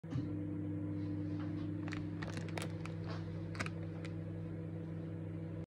Hot 70% bean to bar chocolate frog
Cacao origin: Sambirano Madagascar. Taste profile: citrus fruits, yellow berries, chocolatey